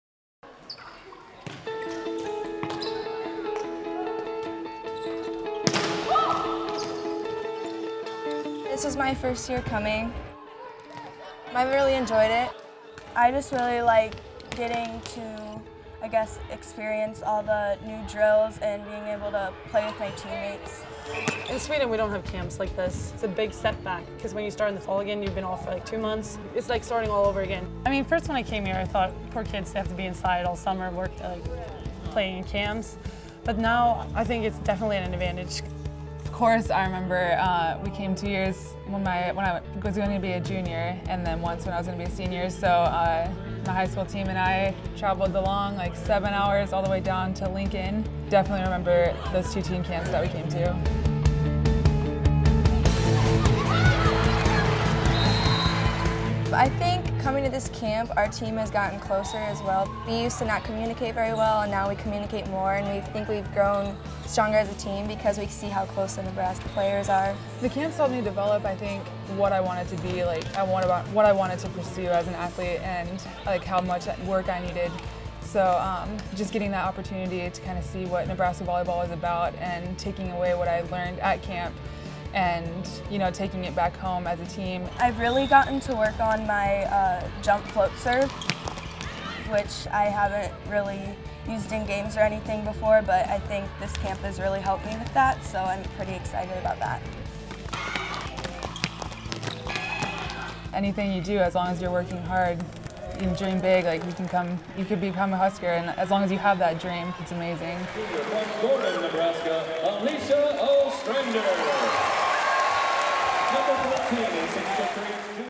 Husker Volleyball: 2014 Volleyball Camps - an NET Sports Feature
Every year the Nebraska Volleyball players become coaches to future Husker hopefuls.